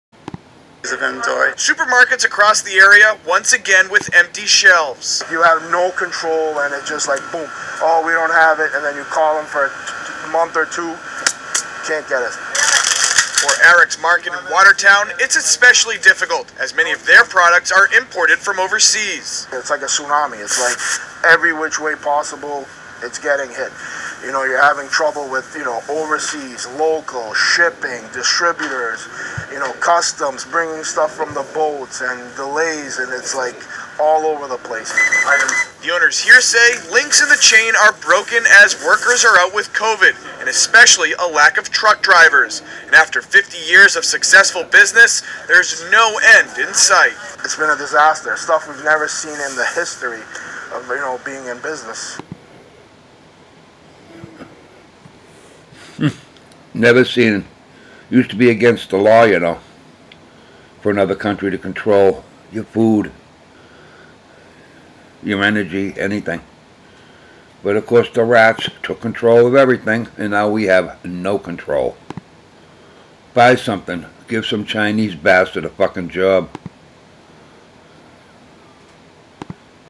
SUPPLY CHAIN We have no control
When you bought something prior to 1980 you gave a American a job. Now you give a job to Asians, Mexicans, South Americans, Africans, and hundred other countries but not the USA.